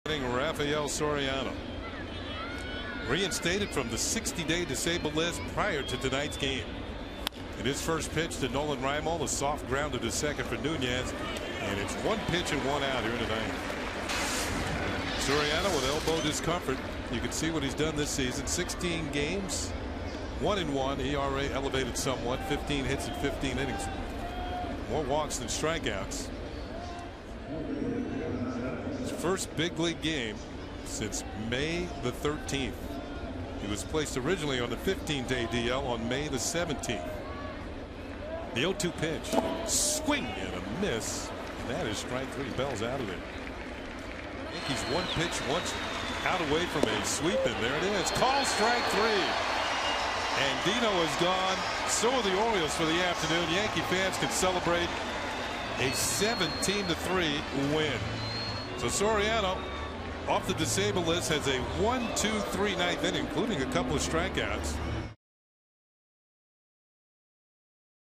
2011/07/30 Soriano returns from DL
BAL@NYY: Soriano returns from DL, hurls perfect ninth